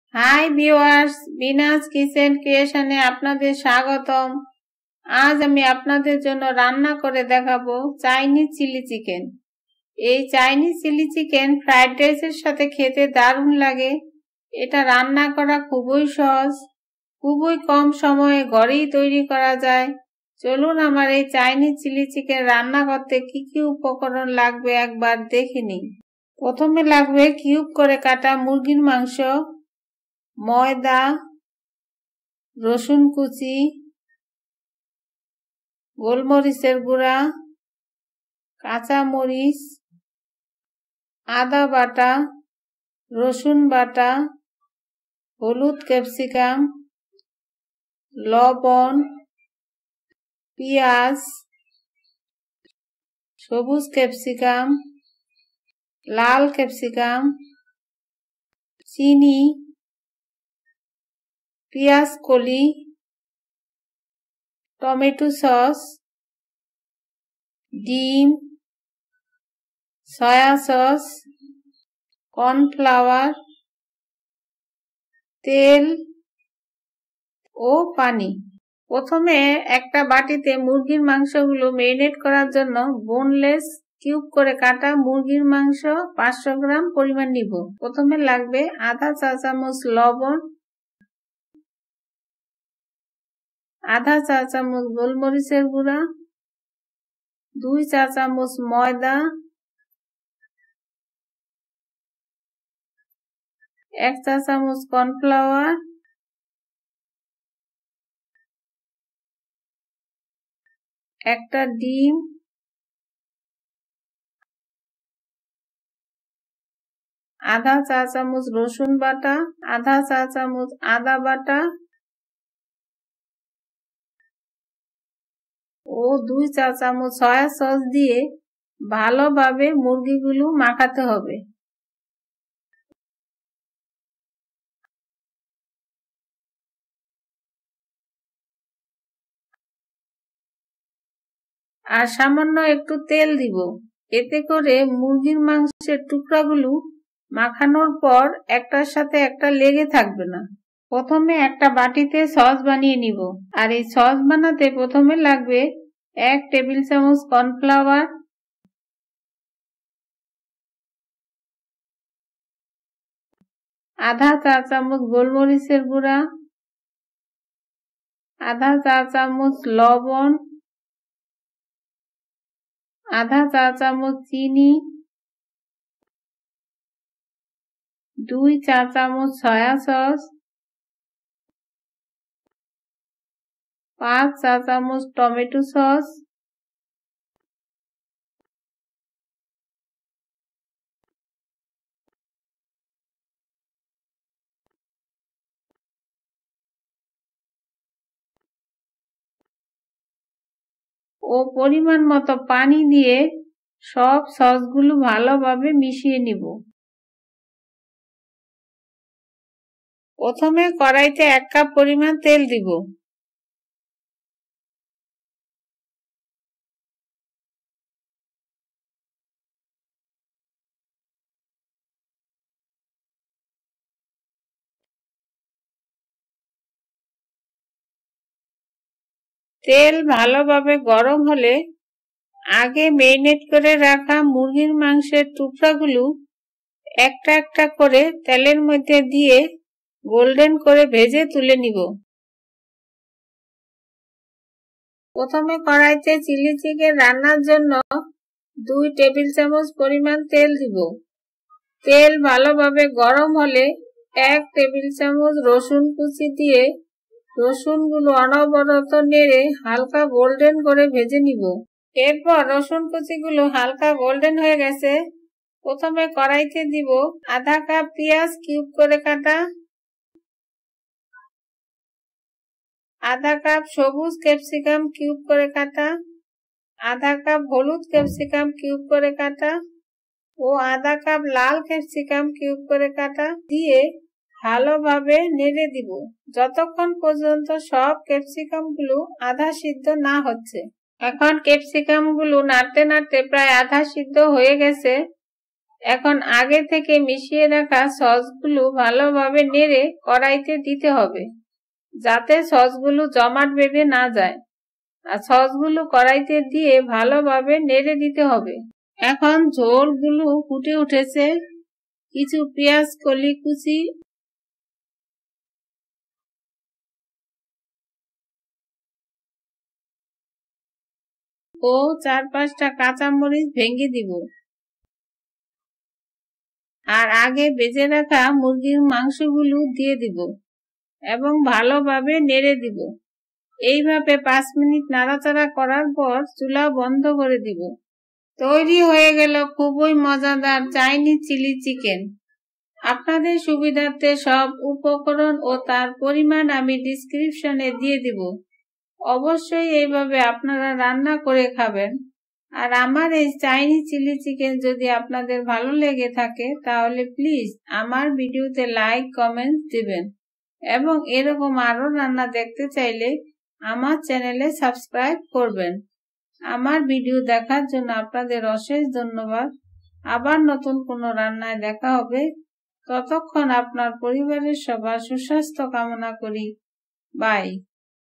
চিলি চিকেন রেসিপি/Bangladeshi/Indian/Chinese Chili/Chilli Chicken/Restaurant Style Chili Chicken
Ingredients:
Chicken: 500 Grams
Flour: 2 tsp
Garlic: 1 tbsp
Black Pepper Powder: 1 tsp
Green Chili: 4/5
Ginger Paste: 1/2 tsp
Garlic Paste: 1/2 tsp
Onion: 1/2 Cup
Yellow Capsicum: 1/2 Cup
Red Capsicum: 1/2 Cup
Green Capsicum: 1/2 Cup
Sugar: 1/2 tsp
Spring Onion: 1/2 Cup
Tomato Sauce: 5 tsp
Soy Sauce: 4 tsp
Egg: 1
Salt: As Per Taste
Oil: 1 Cup
Water: 1 Cup
Cornflour: 1 tsp

উপকরণ:
মুরগির মাংস: ৫০০ গ্রাম
ময়দা: ২ চা চামচ
রসুন কুচি: ১ টেবিল চামচ
কালো মরিচ গুঁড়ো: ১ চা চামচ
কাঁচা মরিচ: ৪/৫ টি
আদা বাটা: ১/২ চা চামচ
রসুন বাটা: ১/২ চা চামচ
পেঁয়াজ: ১/২ কাপ
হলুদ ক্যাপসিকাম: ১/২ কাপ
লাল ক্যাপসিকাম: ১/২ কাপ
সবুজ ক্যাপসিকাম: ১/২ কাপ
চিনি: ১/২ চা চামচ
পেঁয়াজ কোলি: ১/২ কাপ
টমেটো সস: ৫ চা চামচ
সয়া সস: ৪ চা চামচ
ডিম: ১ টি
লবণ: স্বাদ অনুসারে
তেল: ১ কাপ
পানি: ১ কাপ
কর্নফ্লার: ১ চা চামচ